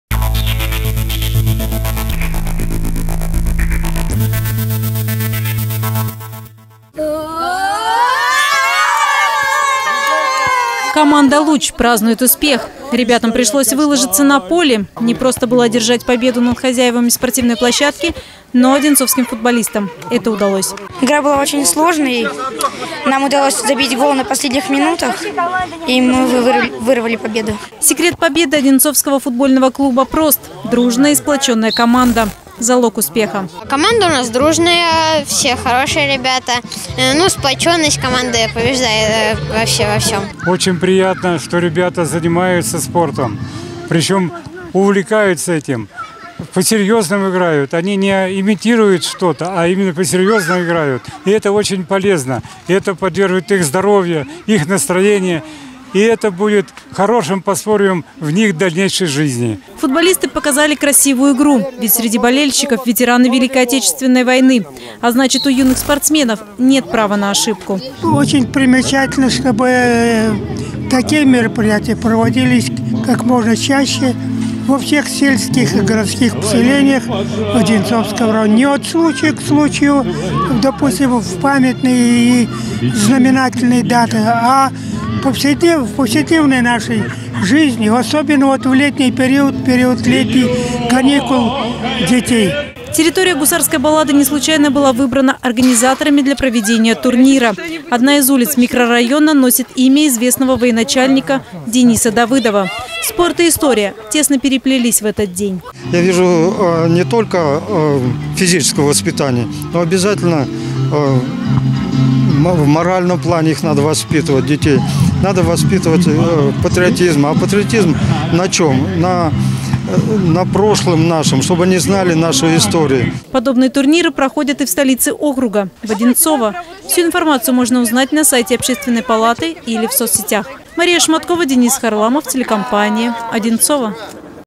Турнир им. Дениса Давыдова
В минувшие выходные в Гусарской балладе кипели настоящие страсти. На местной спортивной площадке прошли сразу два первенства: по мини-футболу и волейболу.